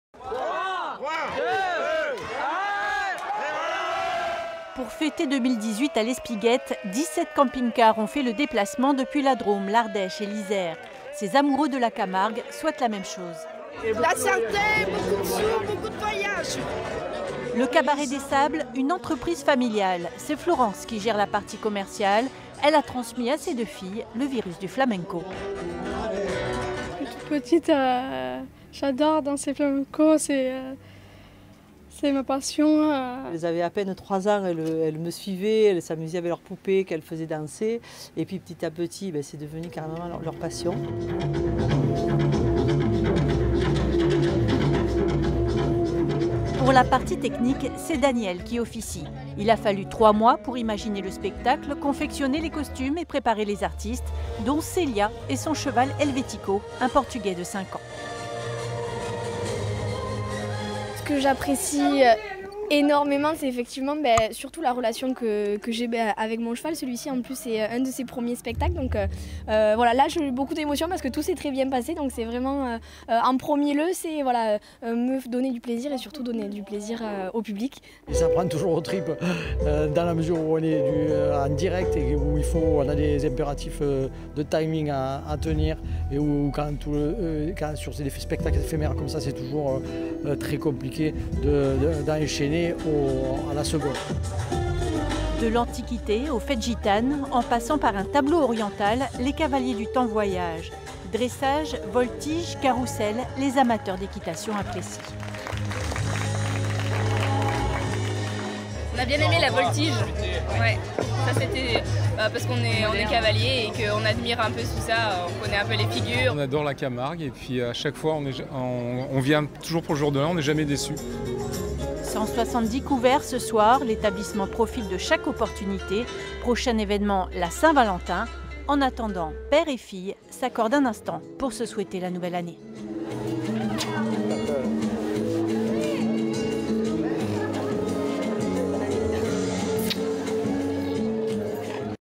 Grau-du-Roi (30) : nouvel an flamenco au milieu des chevaux
Au Grau-du-Roi dans le Gard, les passionnés d'équitation ont échangé leurs voeux 2018 au Cabaret des Sables qui proposait un dîner-spectacle flamenco au milieu des chevaux.

Reportage de Cybèle Plichart et Marie-Laure Lejeune